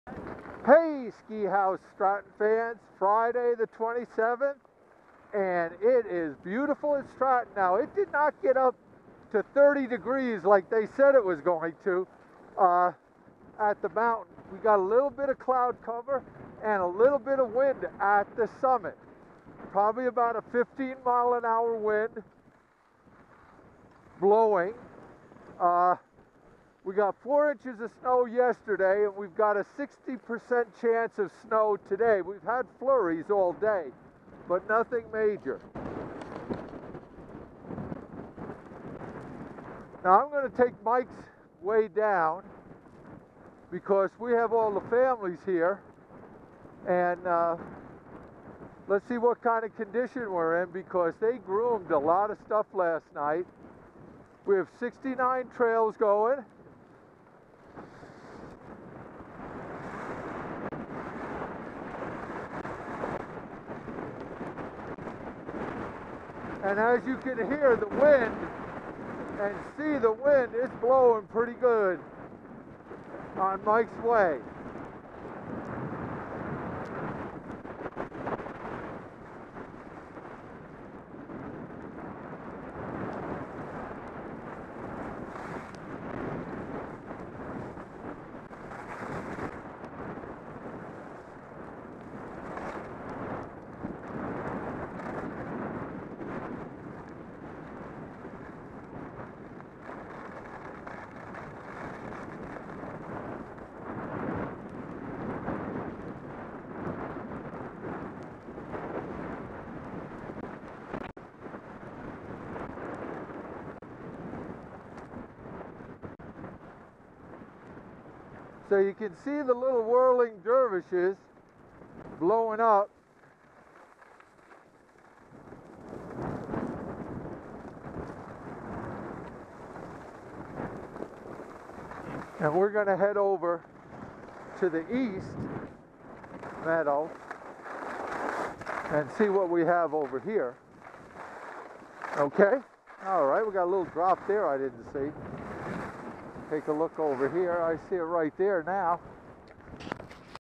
12/27/13 The Big Man afternoon Snow Report from Mikes Way.
Cold temps continue to help Stratton Magic do their thing.